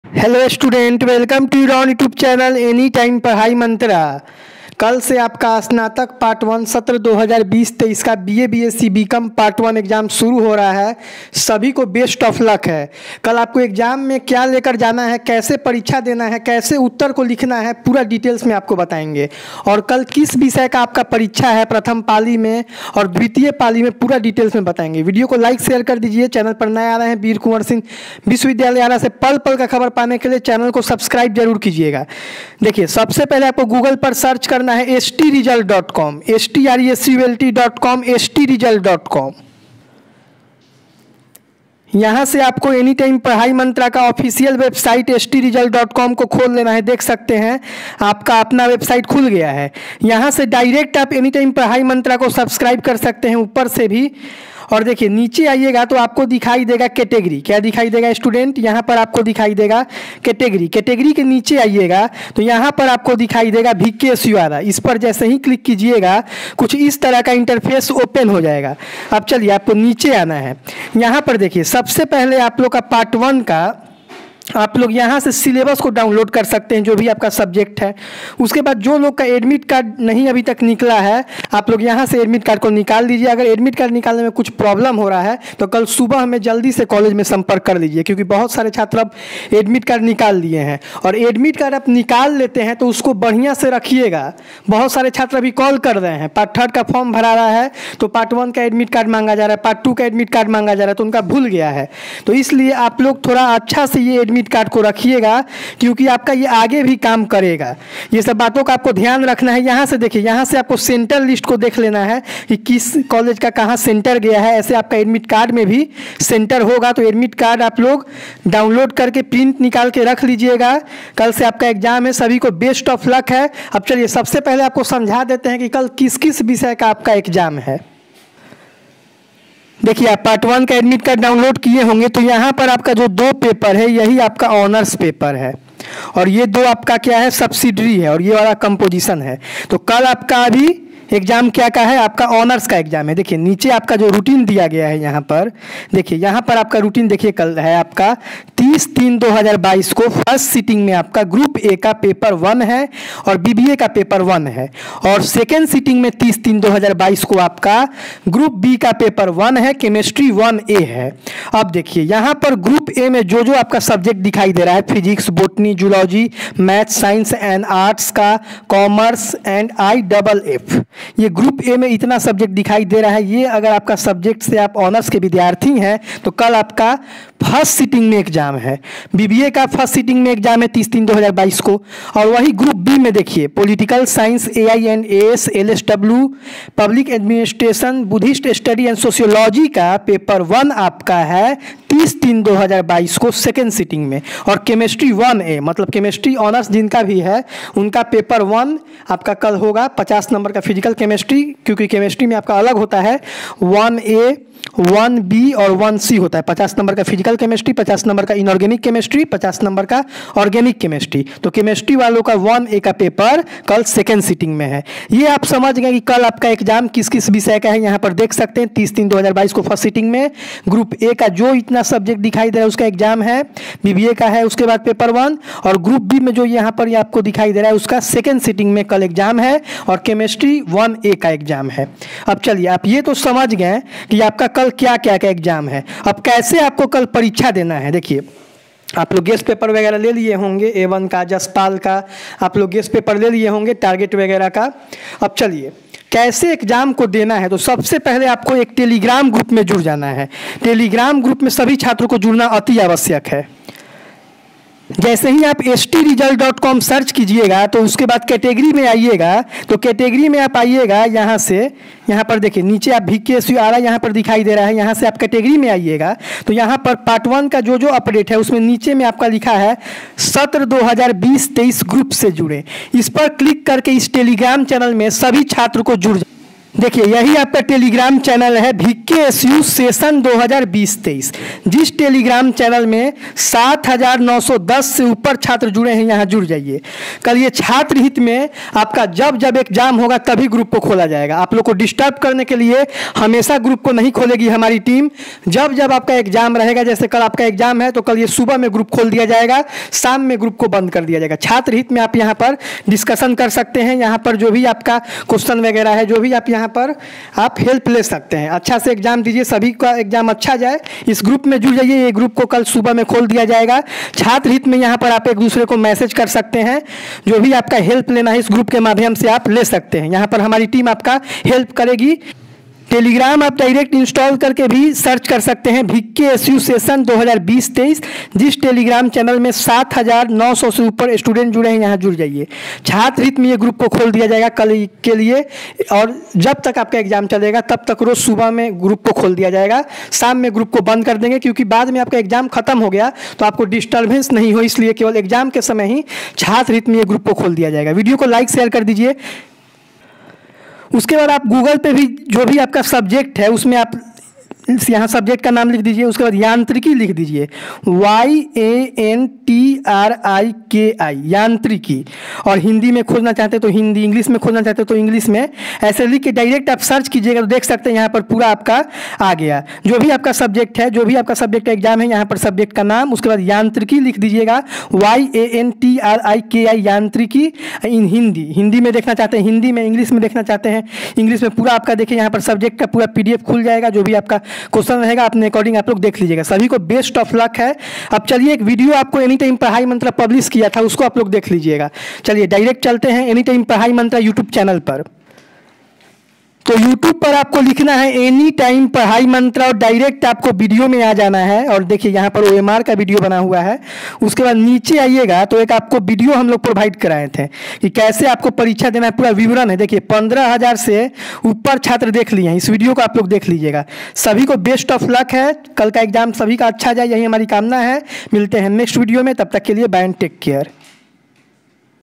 Vksu Part-1 Exam (2020-2023) कल से शुरू । कल किस विषय का Exam है जान लीजिए,यहाँ से उत्तर लिख देना है
V.K.S.U ARA OFFICIAL:
V.K.S.U (सत्र-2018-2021) Special Group में जुड़े सभी छात्र👇👇👇👇👇